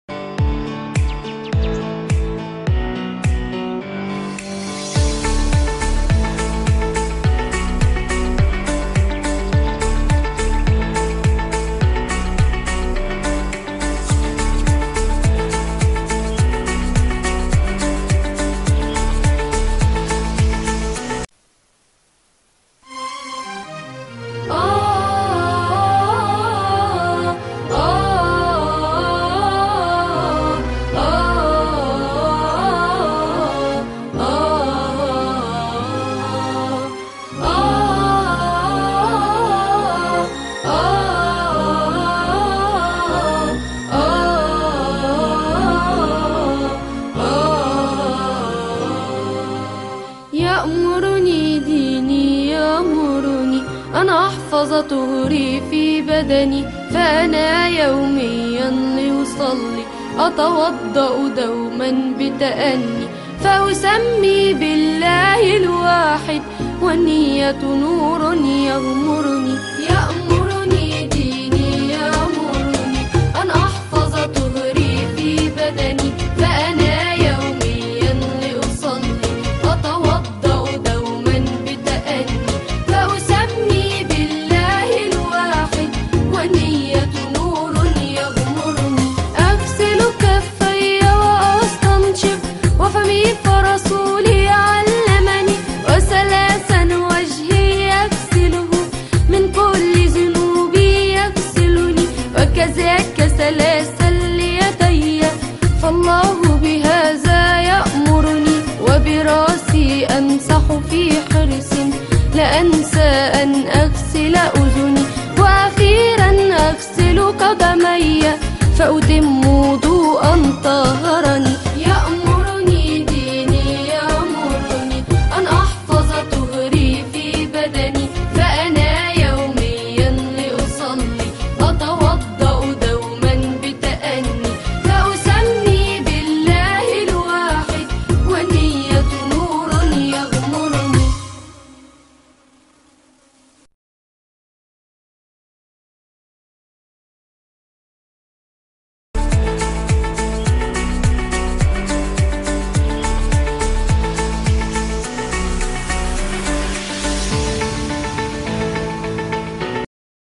نشاط الوضوء والصلاة لمستوى التمهيدي الثاني
تنشئة براعم مؤسسة لورانس على القيم  الدينيةوالإسلامية من أولويات المؤسسة